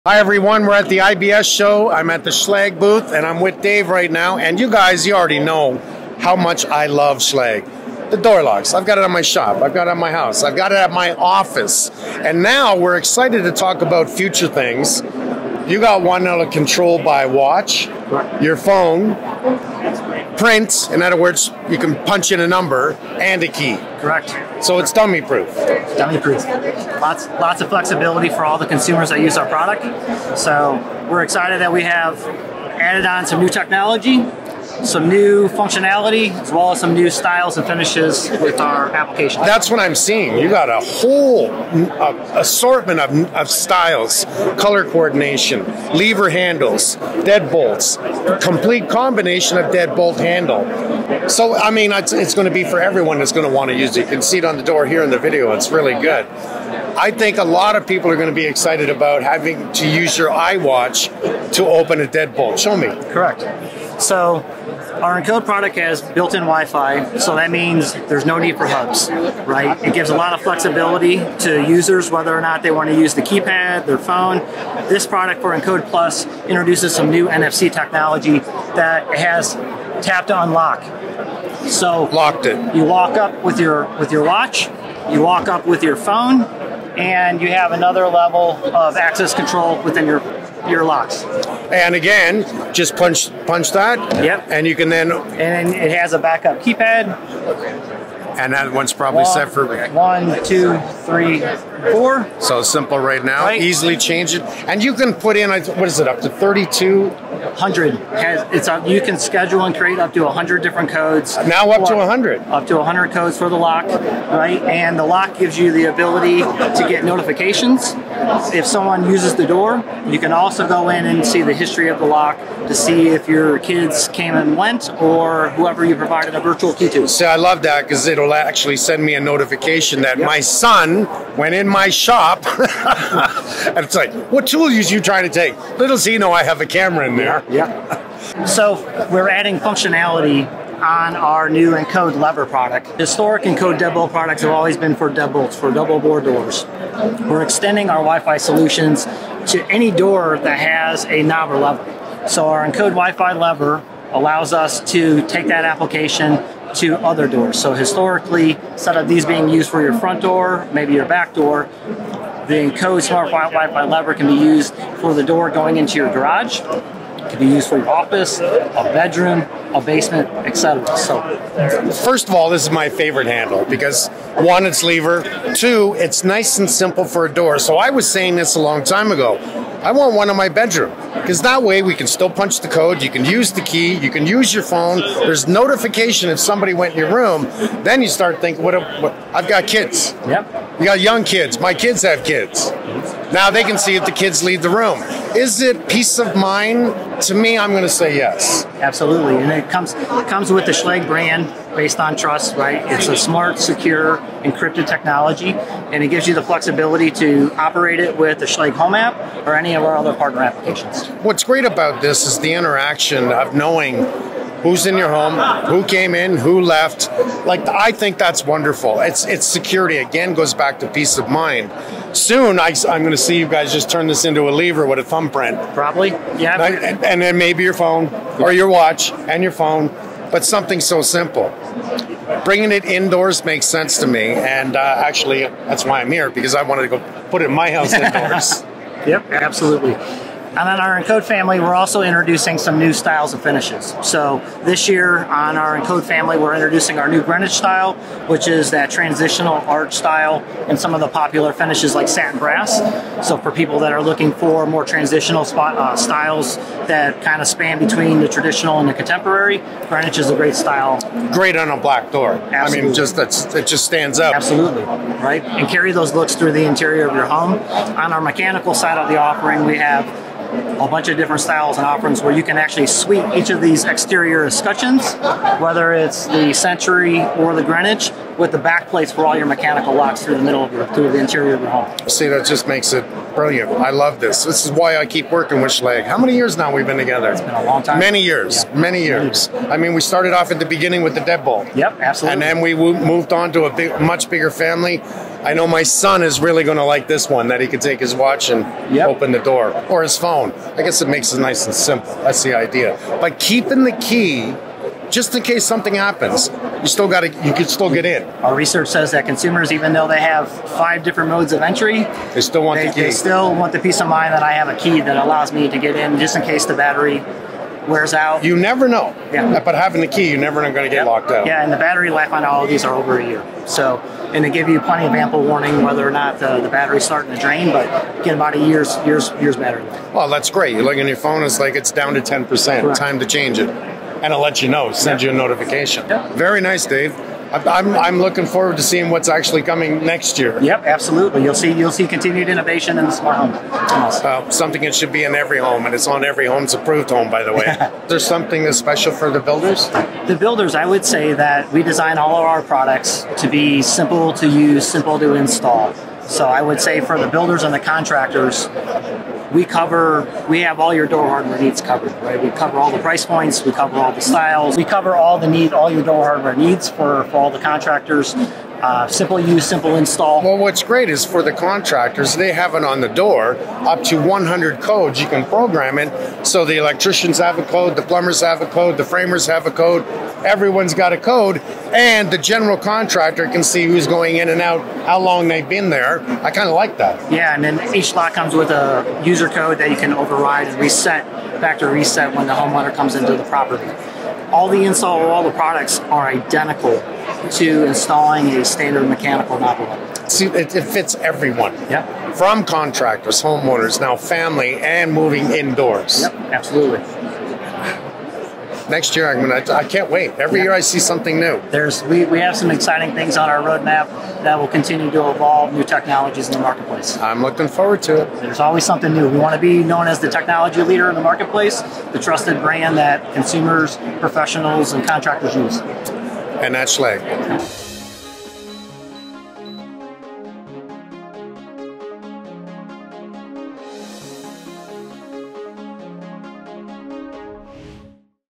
What's New When It Comes To Smart Locks
I stopped by the Schlage Booth at the International Builder’s Show to check out what’s new and exciting. Here's everything they had planned.